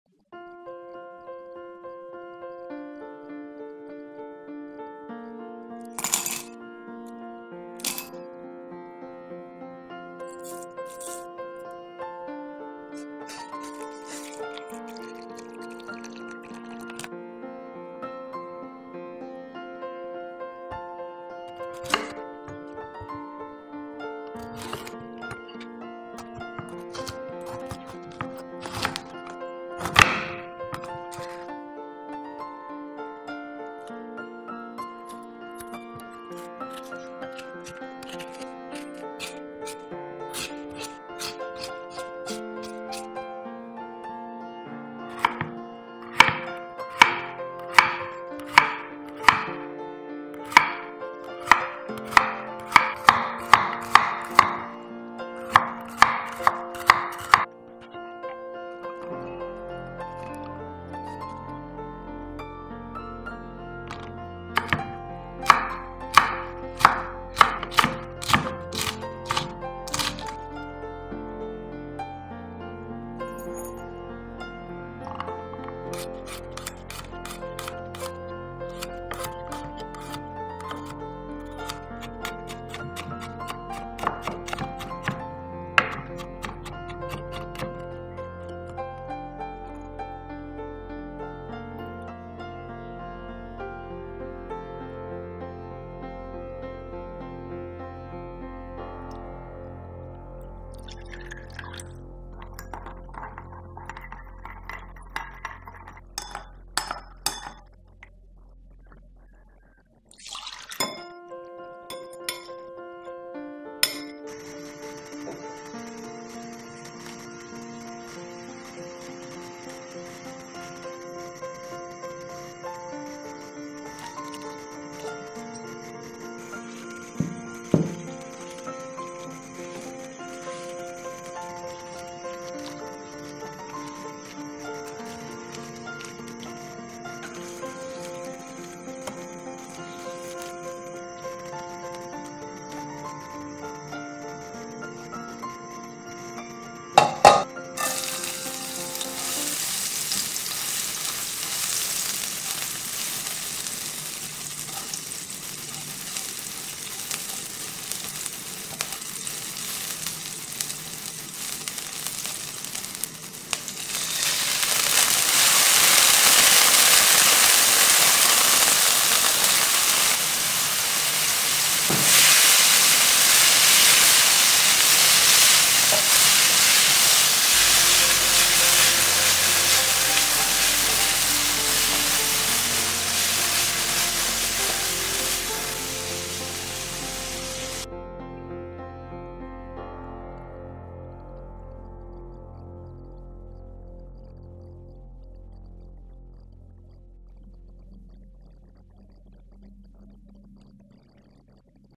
Cooking bitter melon like this is very delicious :: not too bitter and refreshing :: delicious food
Hi everyone, cooking bitter melon like this is very delicious, not too bitter and refreshing, you should try it...

Available in 28 languages, please select subtitle to your language.

Ingredients
1 handful of dried wood ear mushrooms
1 bitter melon
2 cayenne peppers
3 cloves of garlic
1/2 tsp salt
1/2 tsp powdered chicken stock or mushroom stock
1 tsp sugar
1 tsp white vinegar
5 tbsp water
Add 2 tbsp dried shrimp

bitter melon recipe
bitter gourd fry recipe
delicious food